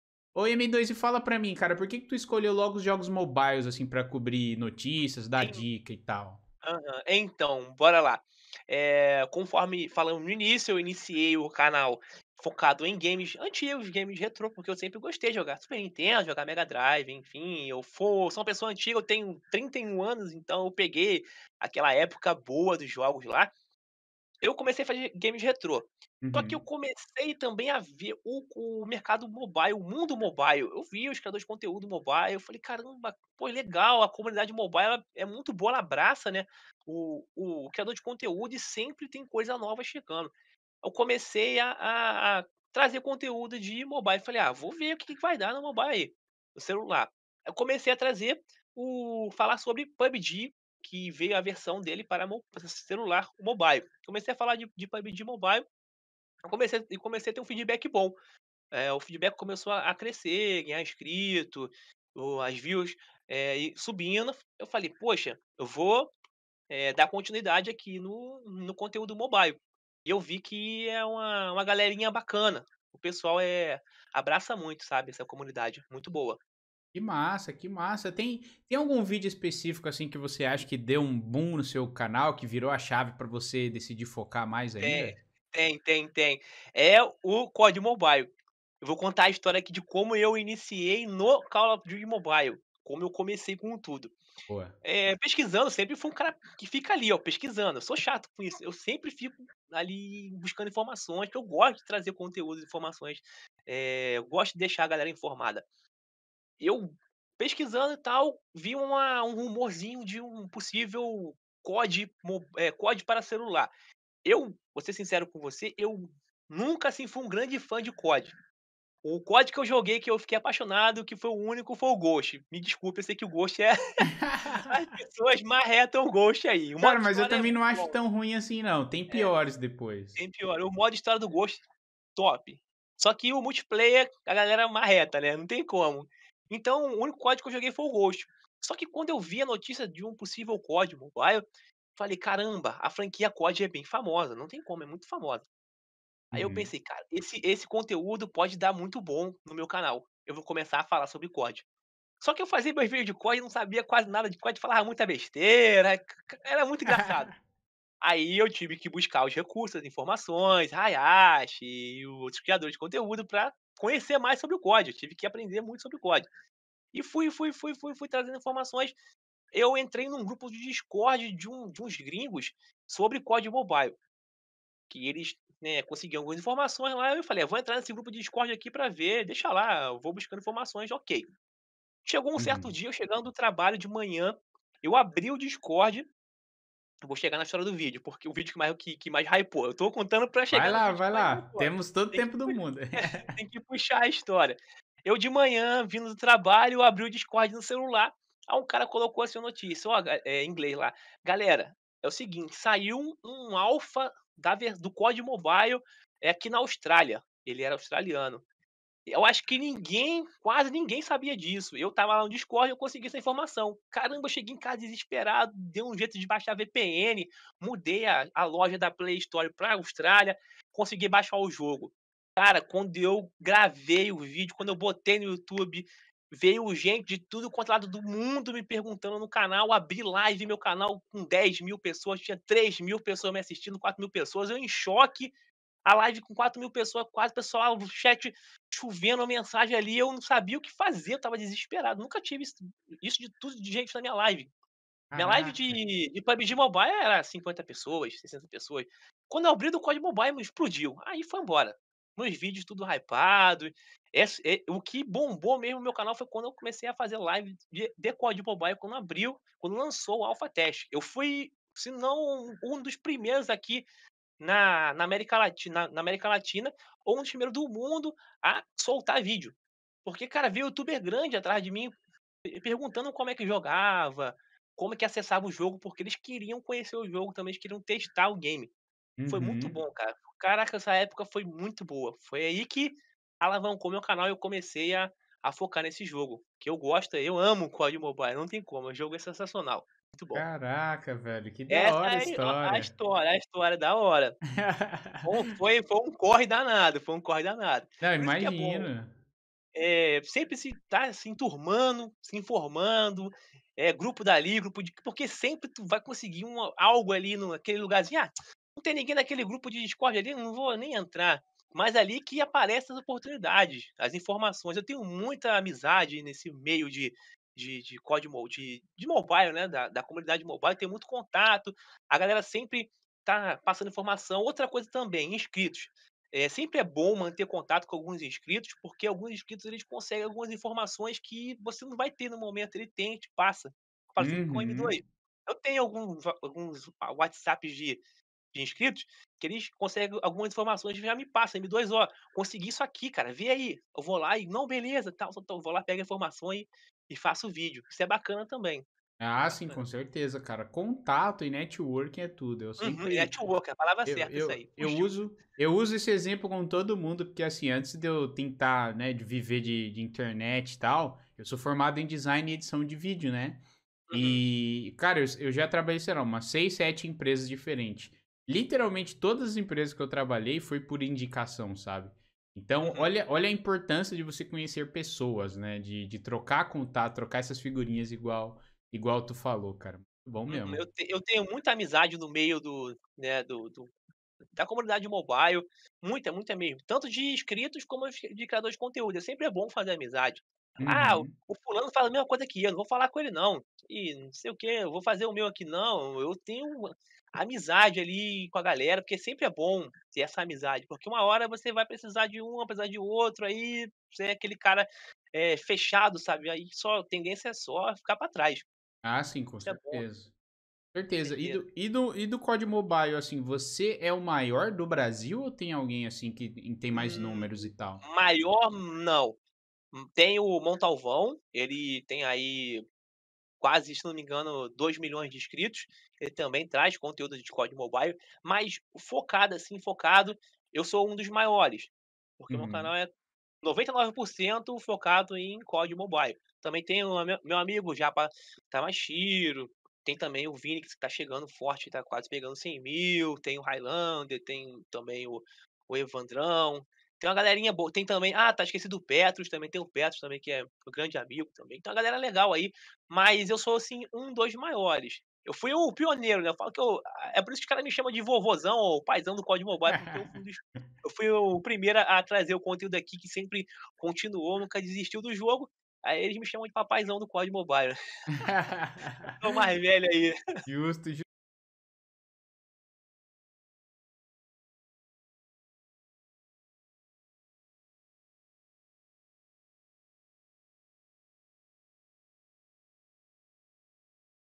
COMO M2GAMES SE TORNOU O PAI DO COD MOBILE
Nesse corte o @m2games nos conta como foi seu início no Youtube e como se apaixonou pelo COD MOBILE. Fala também de suas inspirações e referências no cenário, que fez ele se tornar um dos maiores criadores de conteúdo de COD MOBILE do Brasil! Aperte o play e vem conversar com a gente!

Você curte o Call of Cast? Nos apoie em apoia.se/callofcast e ganhe diversos benefícios!

Siga o apresentador do Call of Cast!